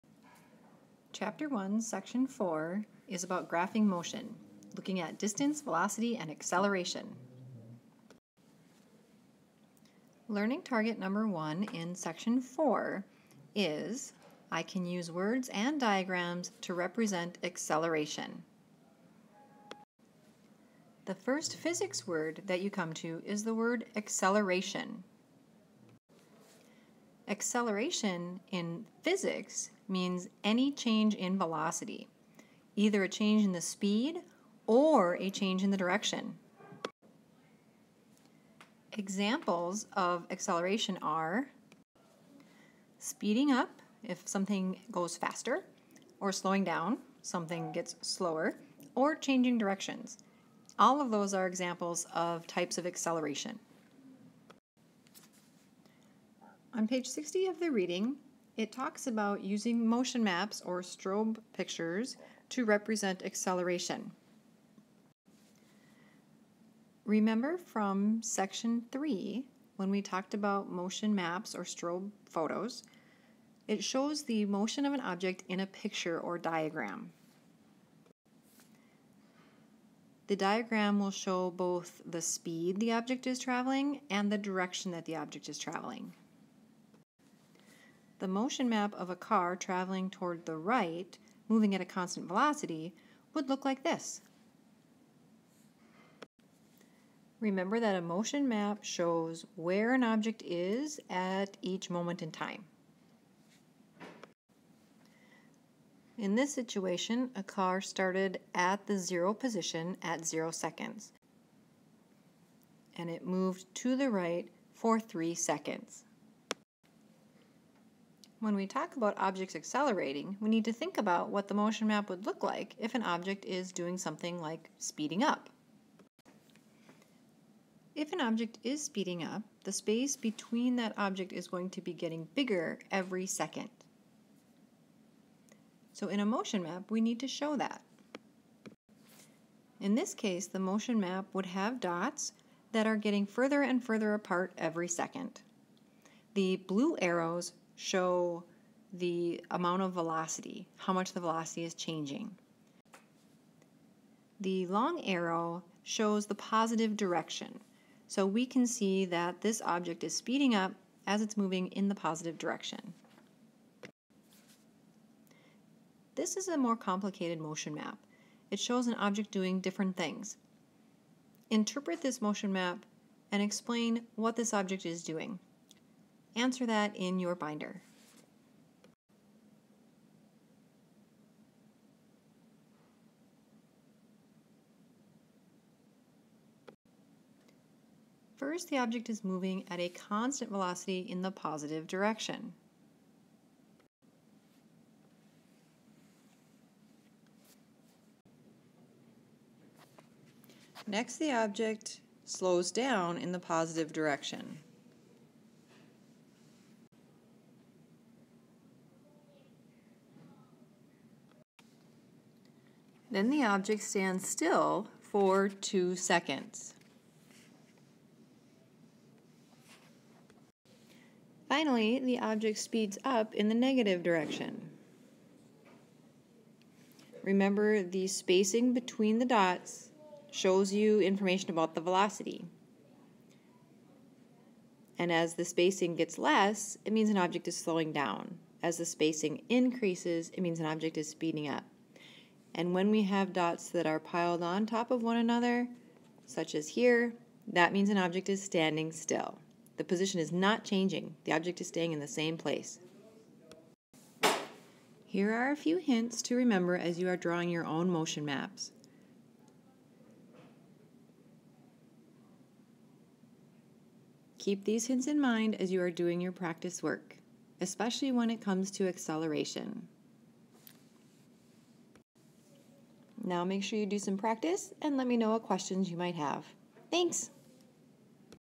Chapter 1, Section 4 - LT 1 Making Motion Maps with Acceleration
Motion Maps of Accelerating Objects - Chapter 1, Section 4 - Graphing Velocity and Acceleration